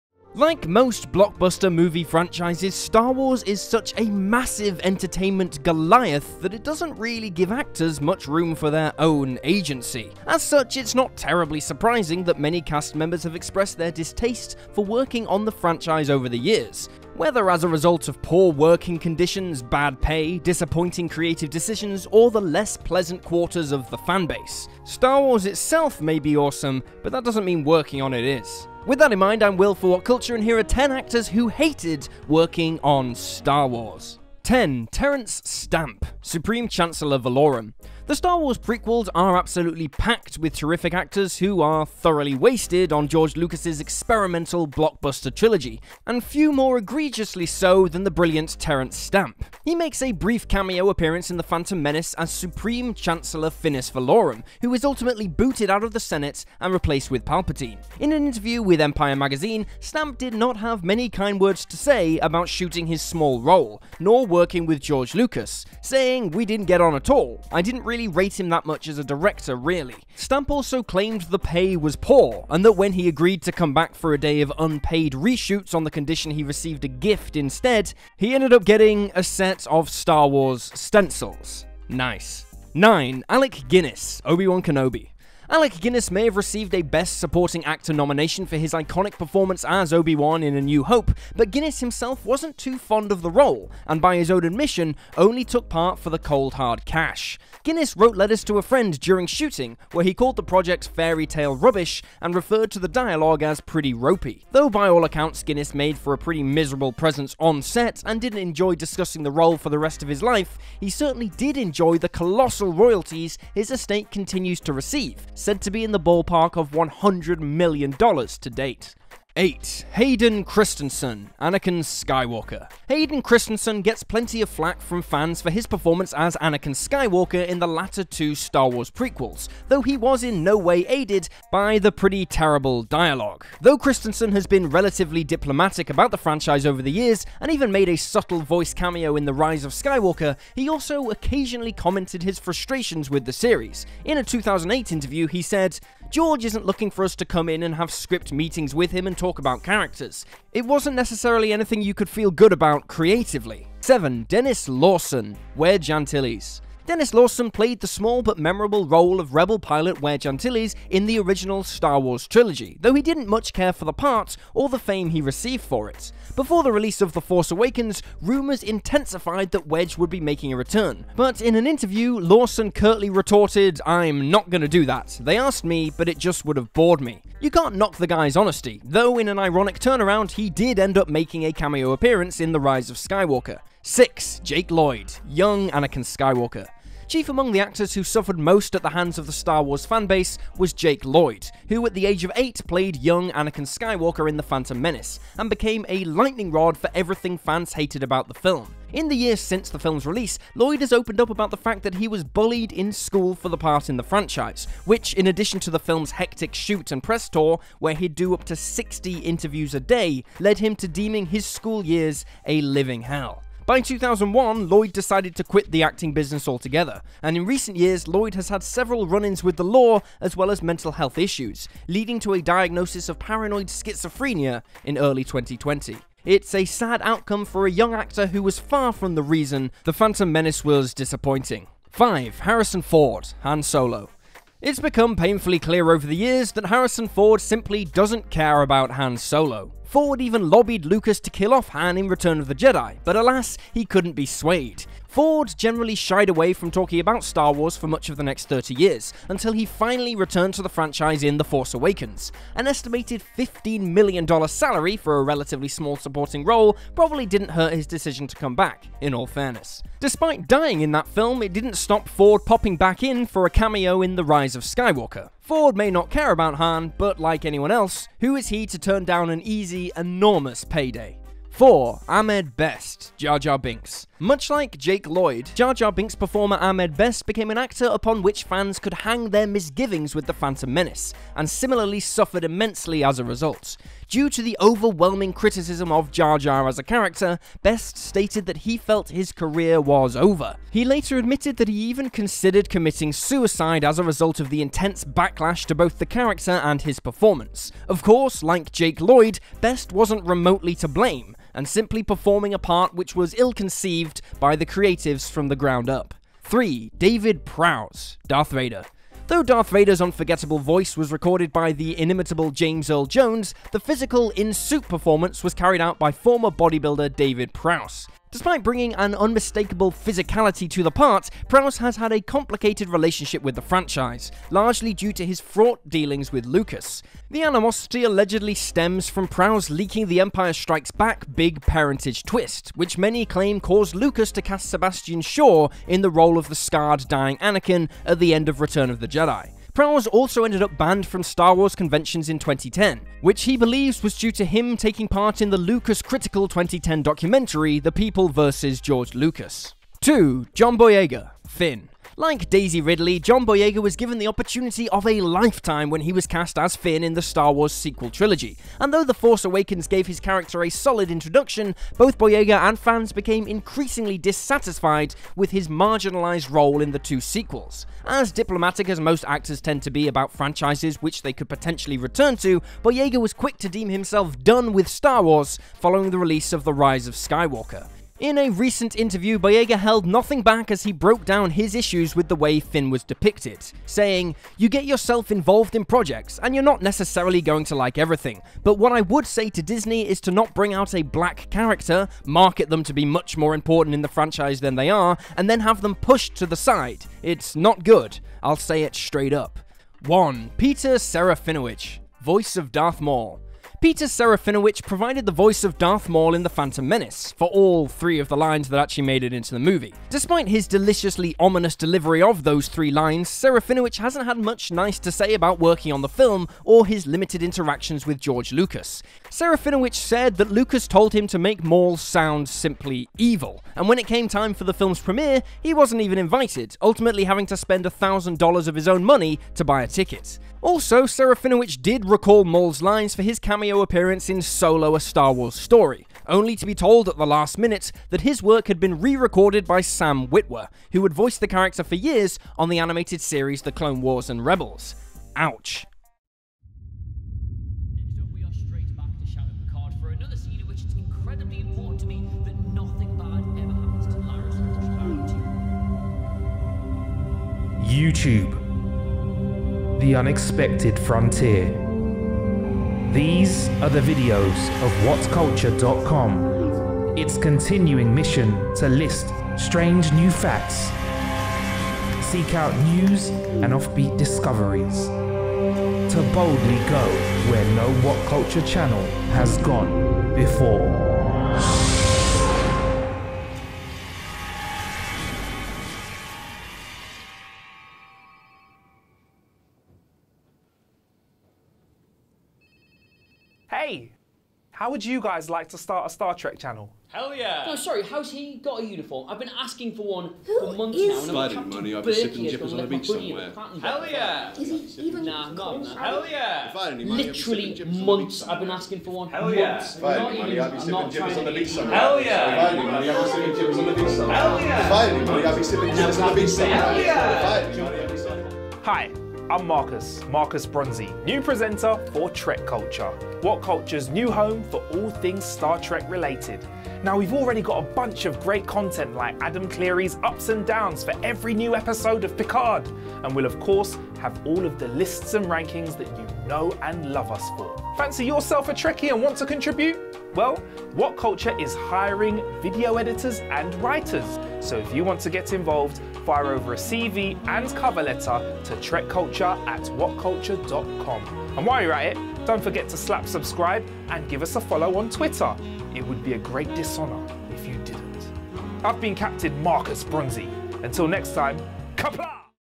10 Actors Who Hated Working On Star Wars
Money isn't everything.